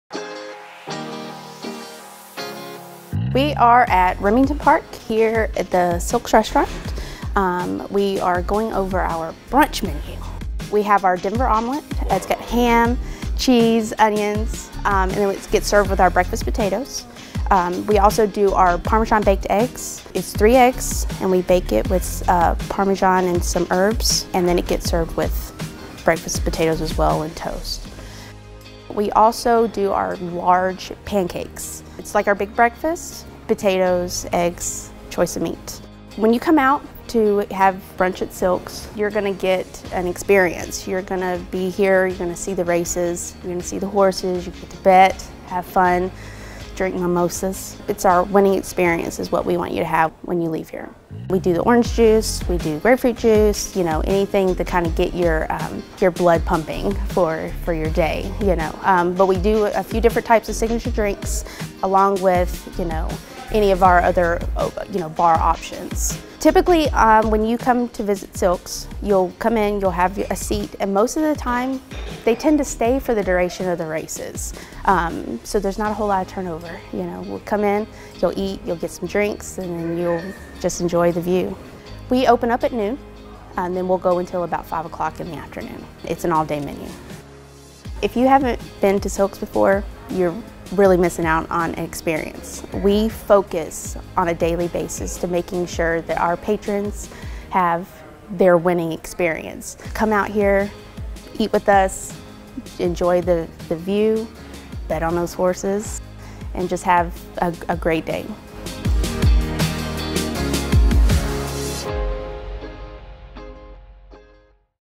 Silks Brunch
Great racing and great dining come together in the Silks Dining Room on the Clubhouse Level. Silks Fine Dining opens at 5:00 p.m. Thursday, Friday and Saturday and Sunday at noon during the Quarter Horse live race days March 9 through June 2.

This ‘a la carte’ restaurant boosts five tiers of fine dining with an outstanding panoramic view of the racetrack and individual television monitors at many of the tables.

Our exceptional menu includes steaks, seafood, salads, chicken, and pastas that will satisfy any appetite. Remember to save room for our luscious desserts.

Reservations are encouraged but not required. $10.00 minimum Food and Beverage purchase required for Adults.